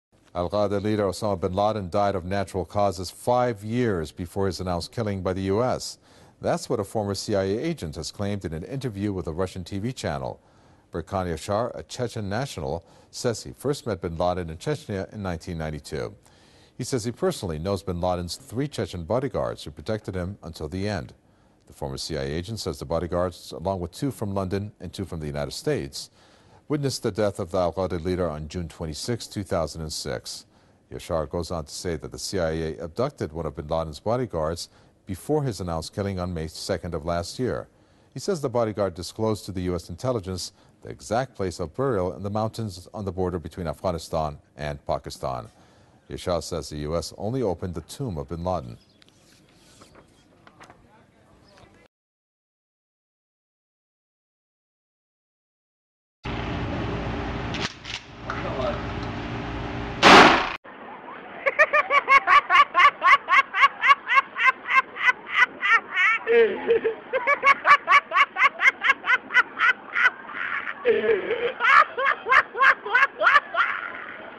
Ex-Agente da CIA: Osama Bin Laden Morreu em 2006. Bin Laden died of natural causes. Zero Dark Thirty
Ex-Agente da CIA: Osama Bin Laden Morreu de Causas Naturais - Osama Bin Laden died of natural causes
The Man Who Killed Osama bin Laden .